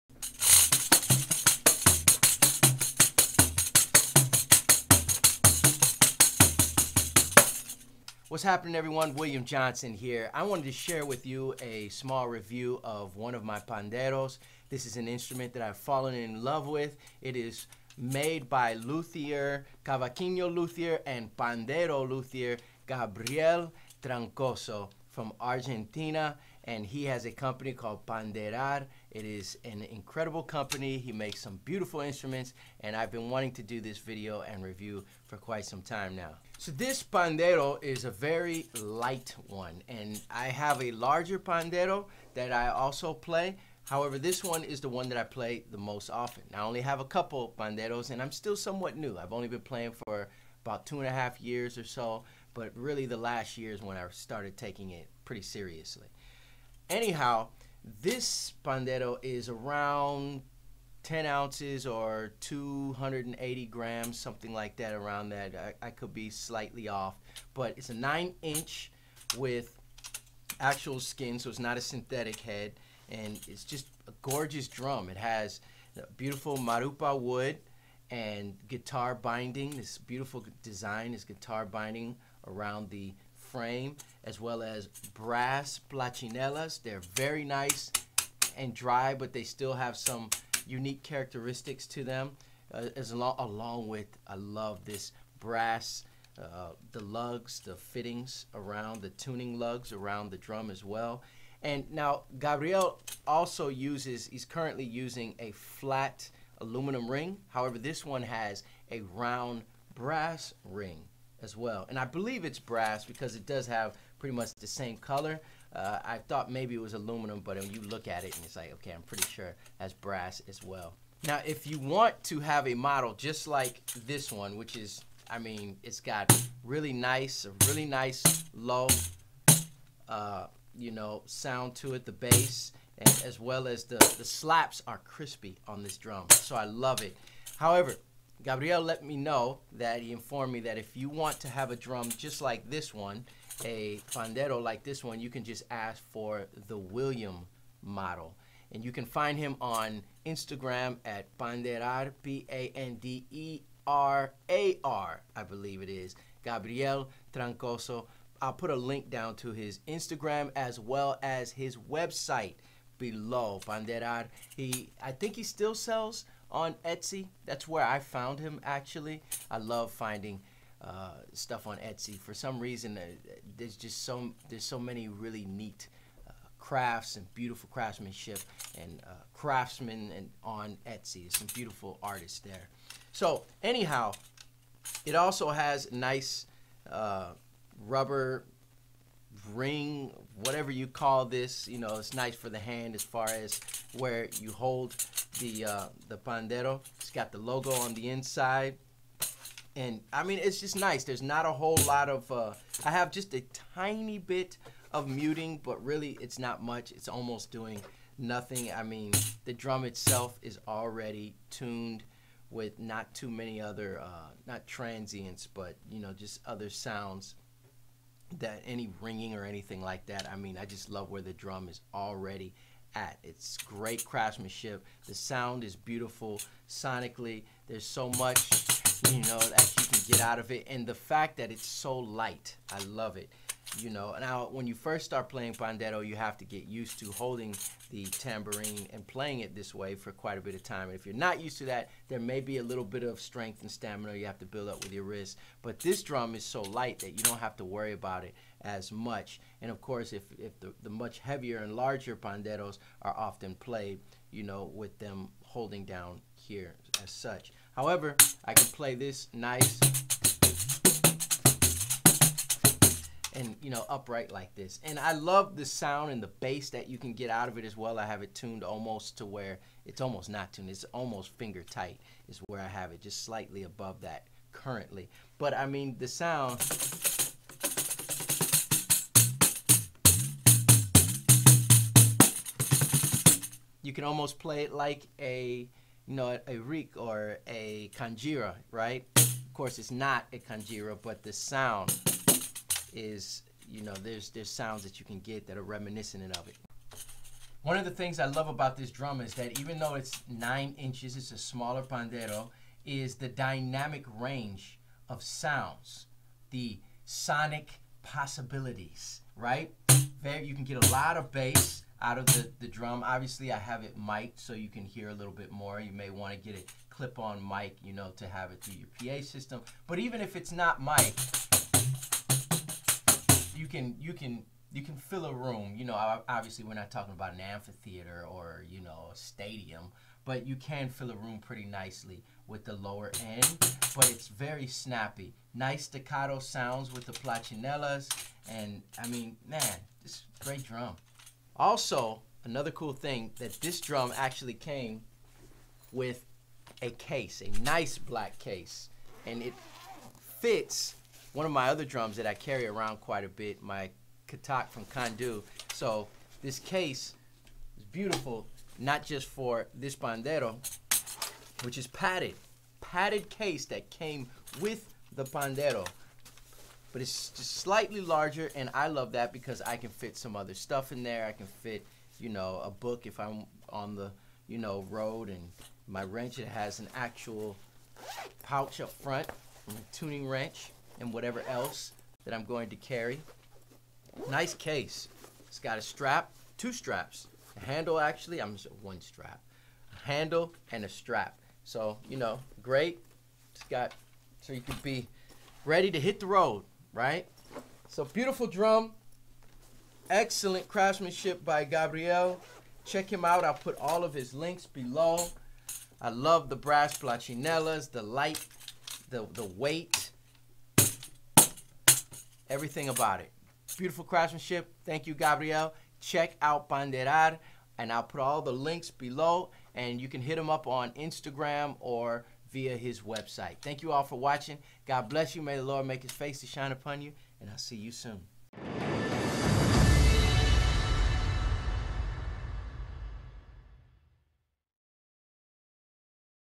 Pandeiro drum; Frame drum review
A short review of a Pandeiro from luthier Gabriel Troncoso of PandeirAR